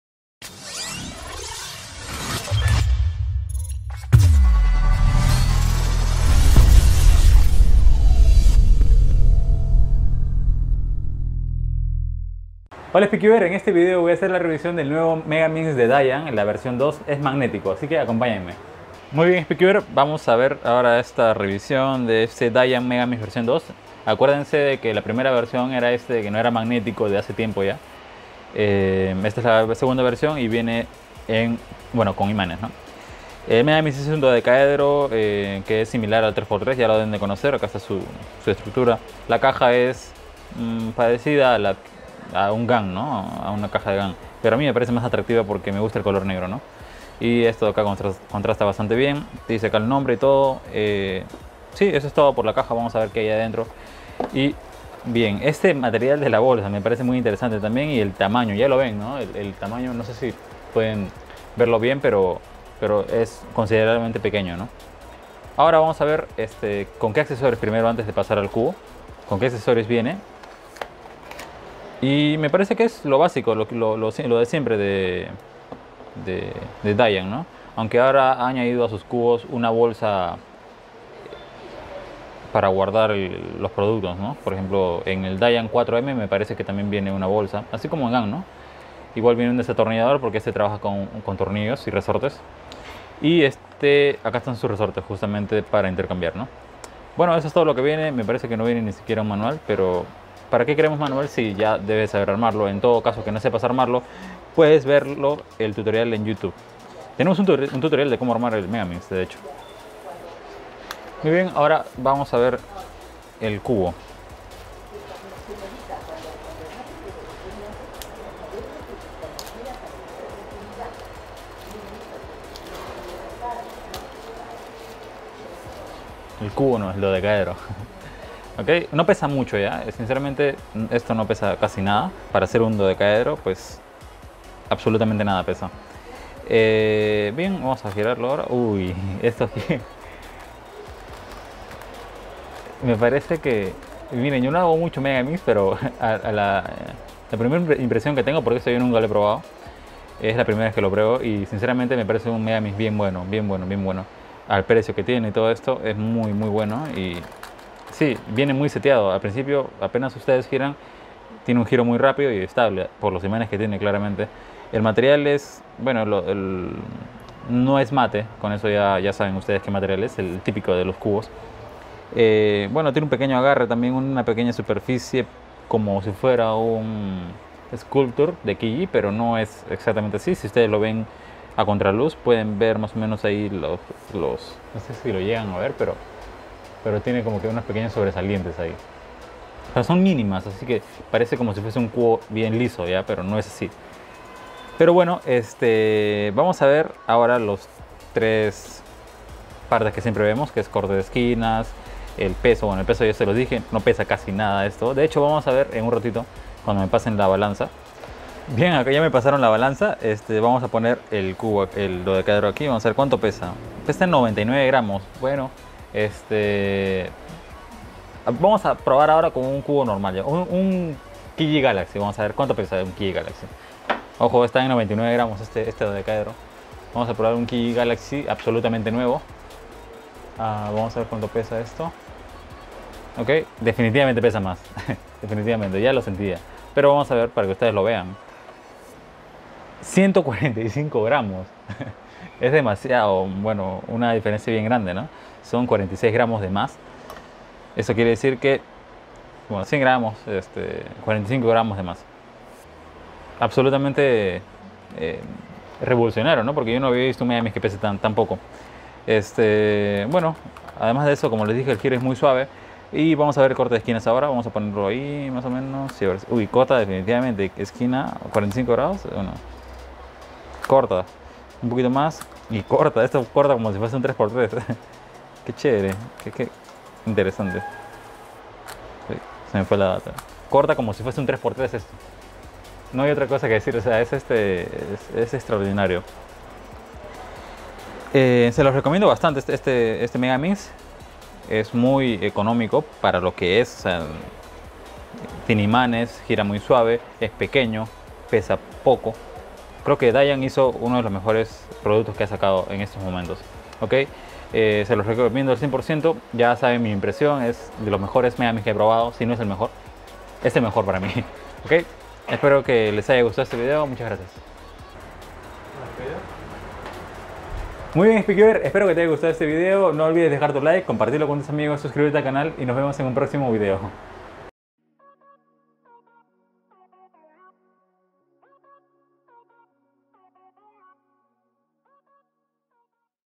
🔥 ¿Nuevo Año, Nuevo Main? 🤩 Review DaYan Megaminx v2 M 😱 | Speedcuber Perú
Tu tienda favorita de cubos te trae un nuevo vídeo, en esta oportunidad es del nuevo megaminx que nos ha traído DaYan, el cuál es el DaYan Megaminx v2 M que hasta ahora es al parecer el mejor megaminx que hemos girado hasta el momento